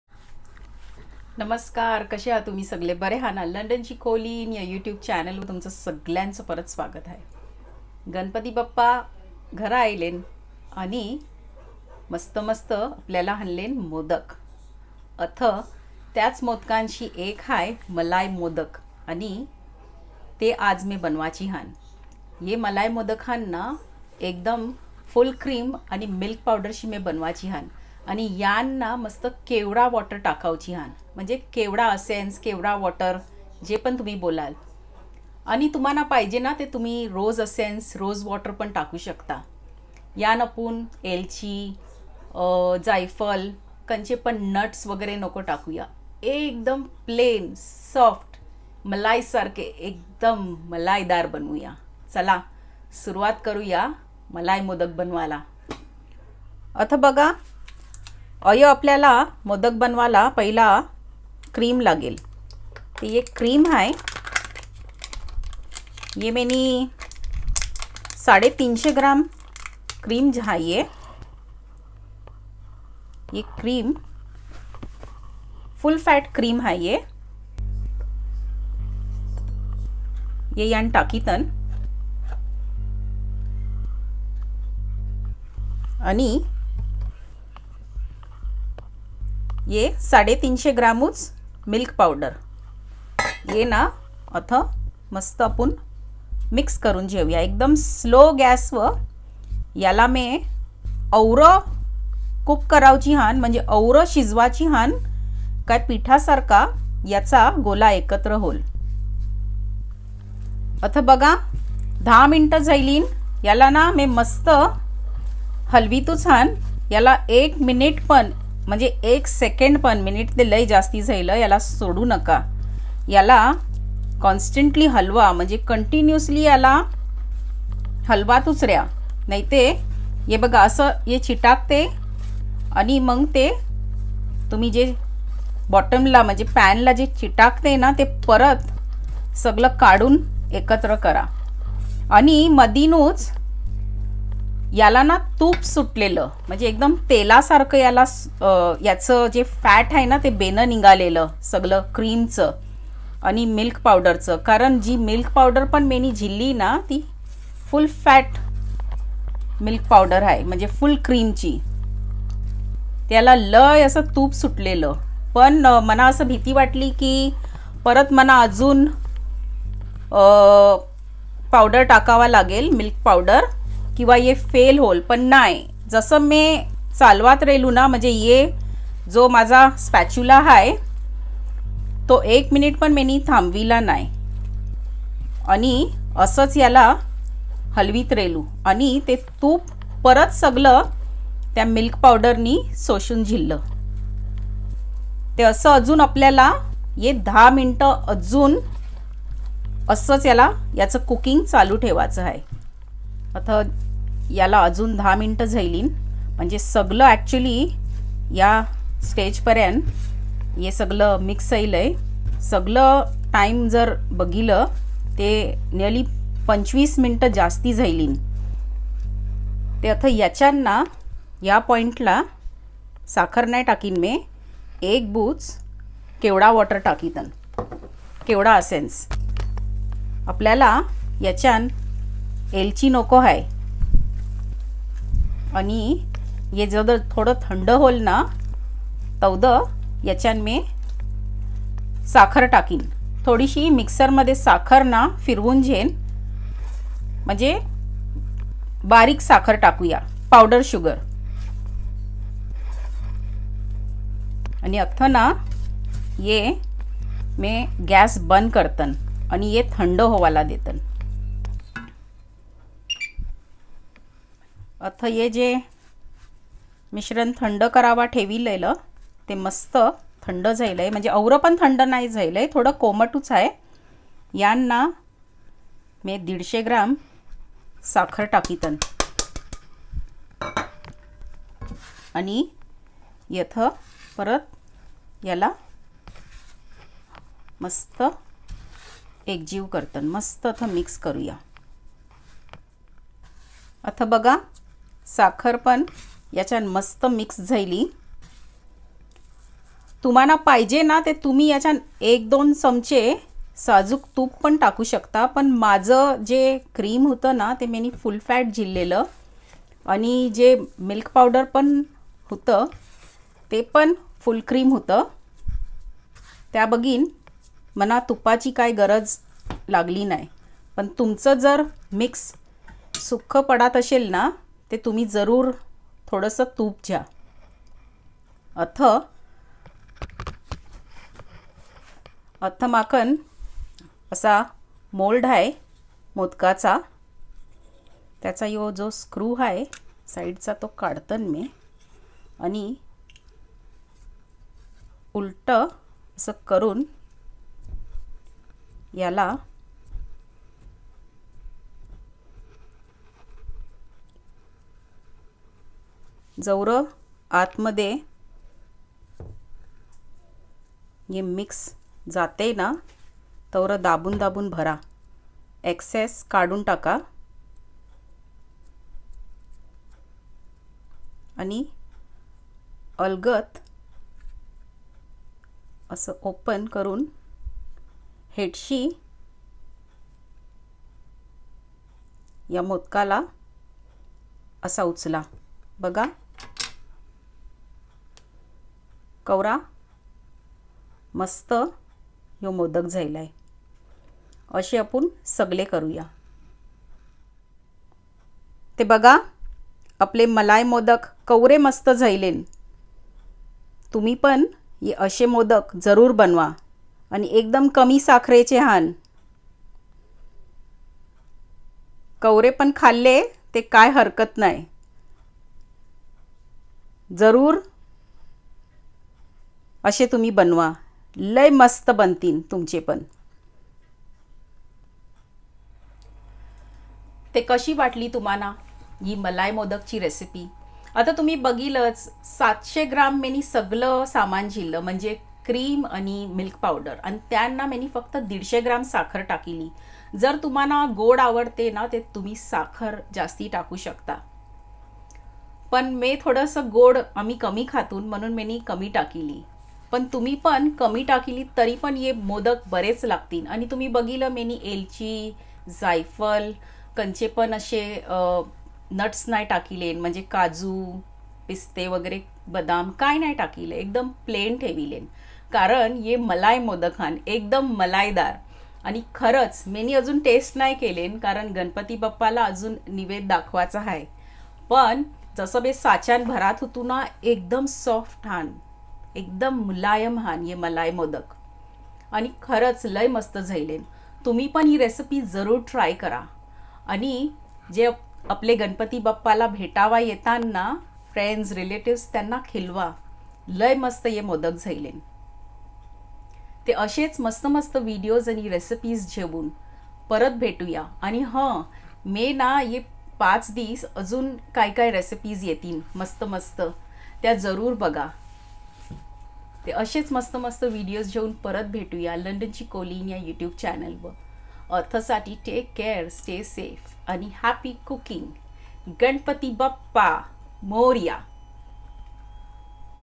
मलाई मोदक GANESH UTSAV SPECIAL || MALAI MODAK
Happy Ganesh Utsav to everyone

This is a very simple malai modak recipe for which we need

- 350 gms milk powder
- 350 ml of full fat fresh cream
- 150 gms
- Few drops of Kewra water